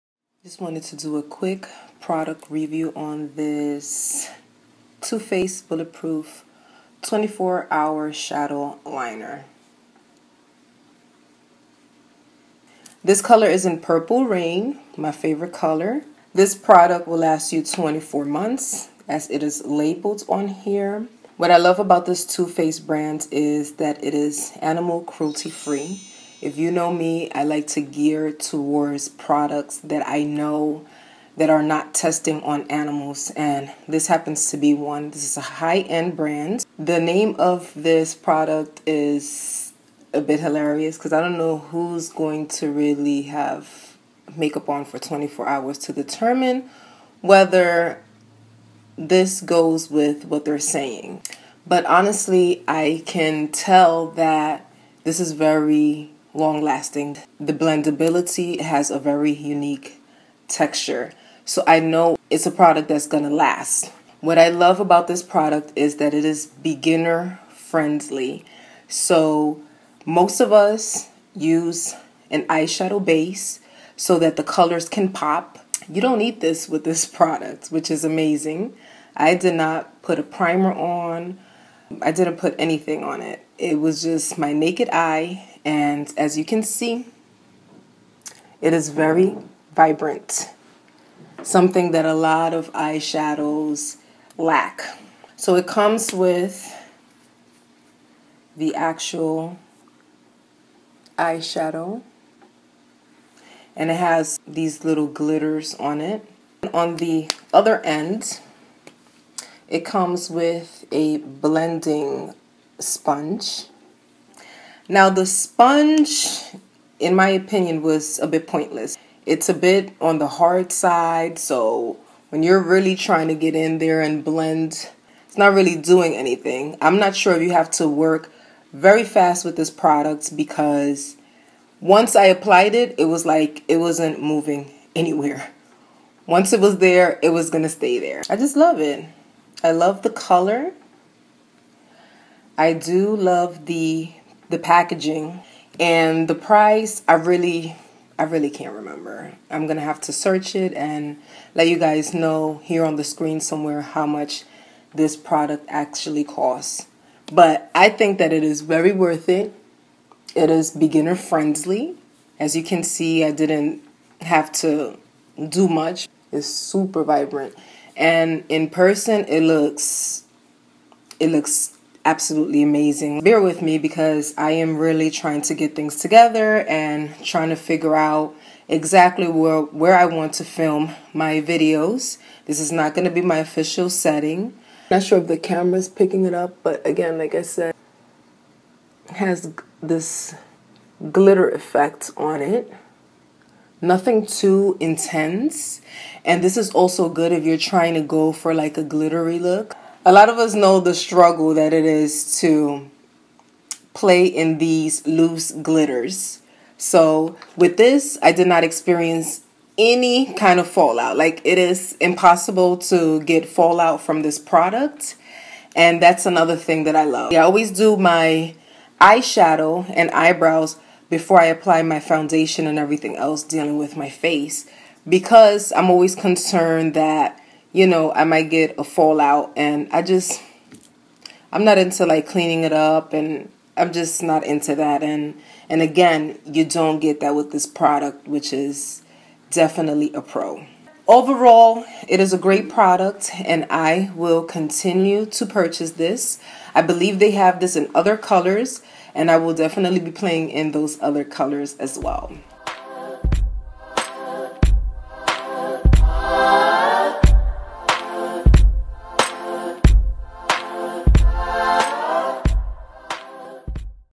Too Faced Bulletproof 24-Hour Eyeliner |Product Review
In this video I share my experience with this product. You can purchase online or in stores like Sephora etc. Prices do vary depending on where you make your purchase. Hope you all enjoyed my mini review

You can also find me here:
Natural hair IG: @Vannie3000
Vegan Lifestyle & Fitness IG: @Alkiefitness
FB: Vannie C's Hair Journey